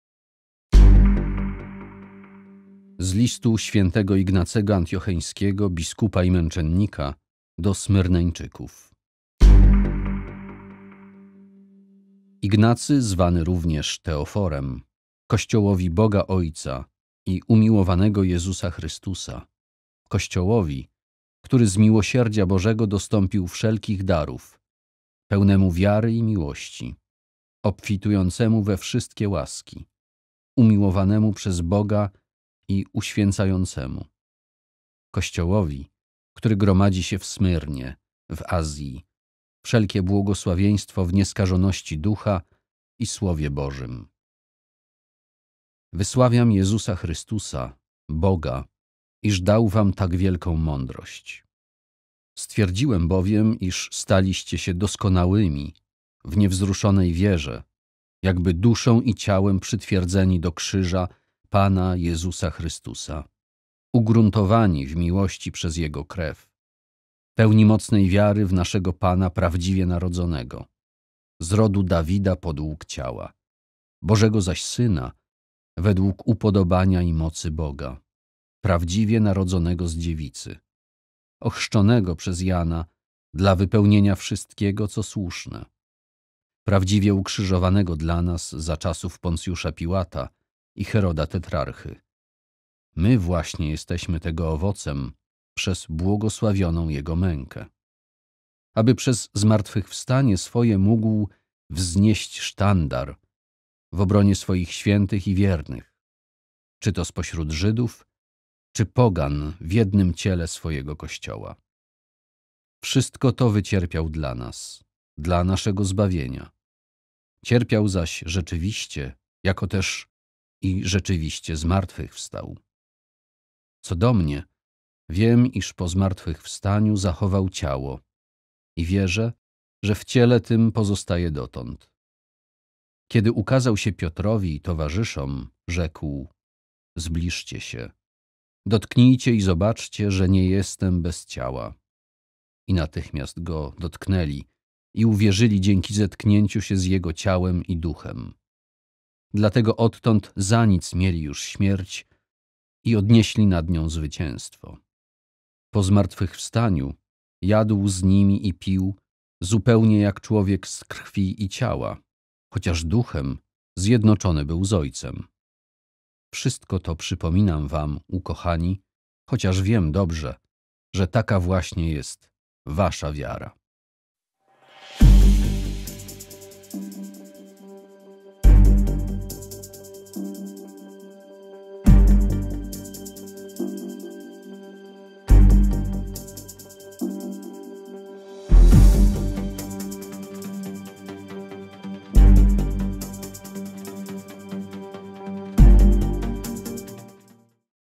GodzinaCzytań | II Czytanie | 29 stycznia 2023
29 stycznia 2023
IV niedziela zwykła

GodzinaCzytań | II Czytanie
Z listu św. Ignacego Antiocheńskiego, biskupa i męczennika, do Smyrneńczyków
Nr 1, 1 - 4, 1